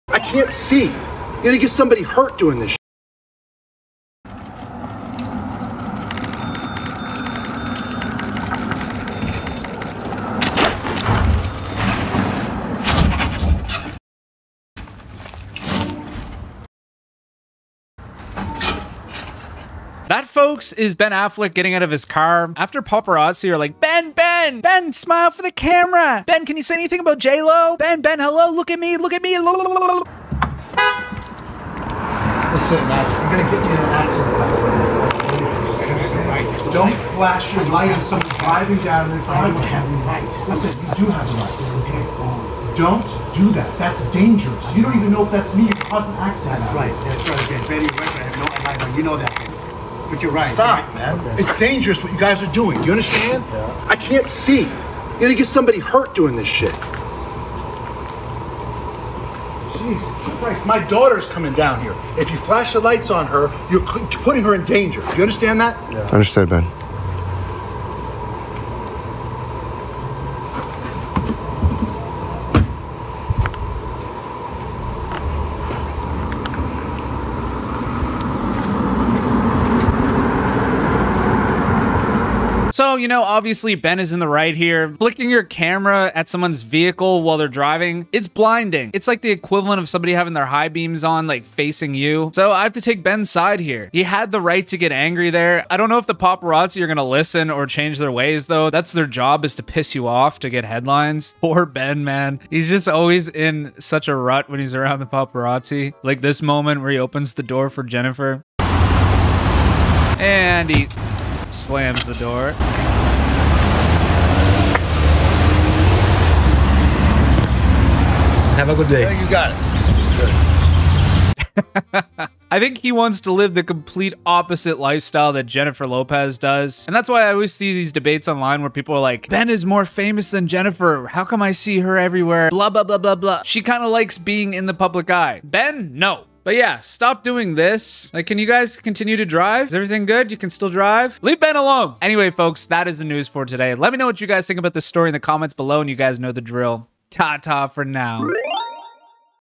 Ben Affleck GOES OFF on paparazzi for being 'dangerous'
In this video, we're discussing Ben Affleck GOES OFF on paparazzi for being 'dangerous'